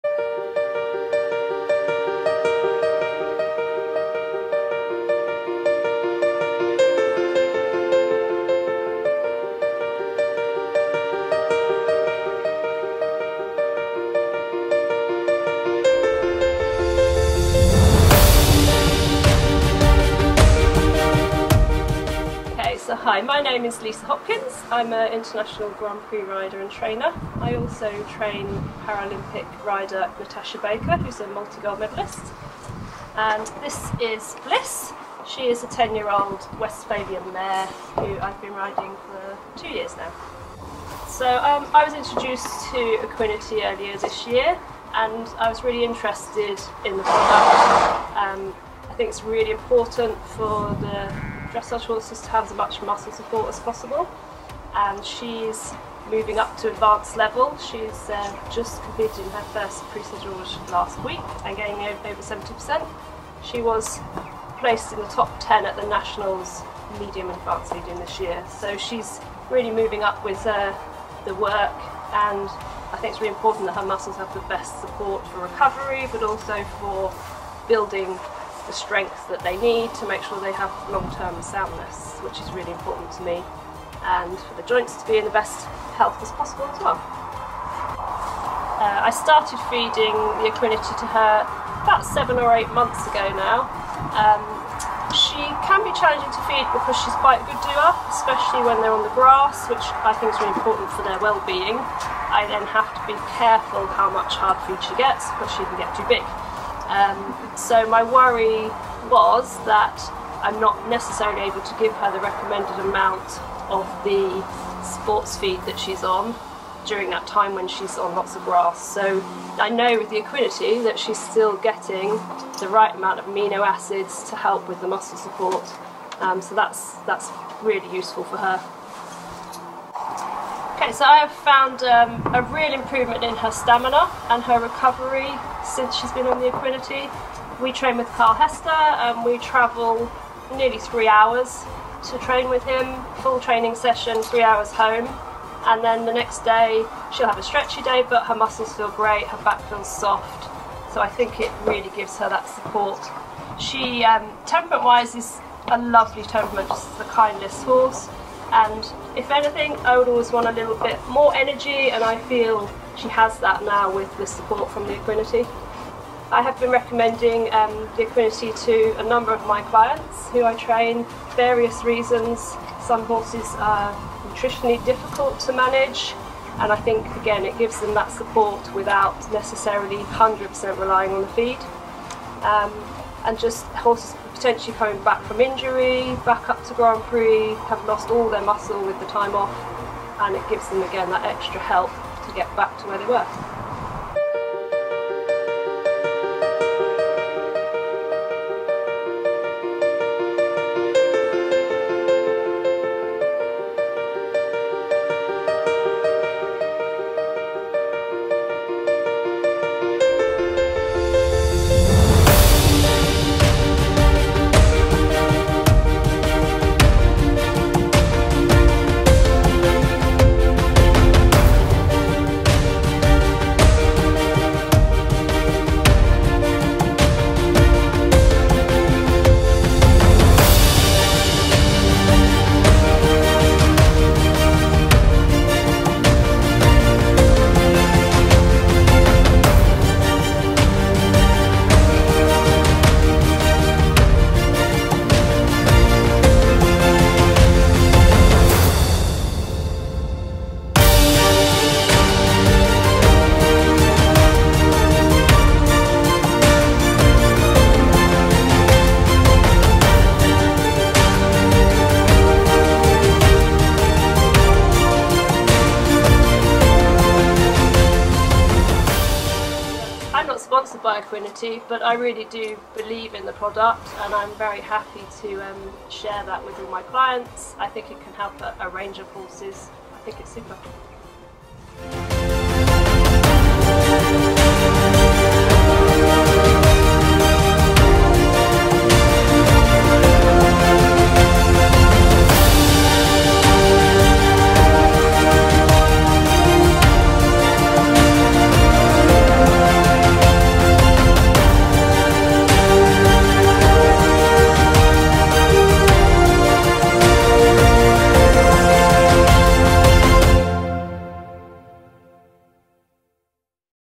Equinety Episode XII - Lisa Hopkins, International Grand Prix Rider and Paralympic Coach
Lisa Hopkins, International Grand Prix rider and Paralympic Coach
Equinety Horse XL - Real Riders Episode XII

Lisa has won dressage titles at International and National level; competing at the Nationals for many, many years and with lots of different horses. She is also a Paralympic Coach, training multiple Paralympic Gold Medalist, Natasha Baker MBE; and has trained with Carl Hester since 2004, arguably one of the greatest dressage riders of a generation.

Lisa was introduced to Equinety Horse XL at the start of 2021 and has seen real benefits with Bliss who she trains and competes through the levels. After seeing such great results, she has continued to feed Equinety since then and has even introduced it to her own clients and friends.

Would your horse benefit from our product?
Also, please visit us on Facebook - simply look up Equinety UK & Europe.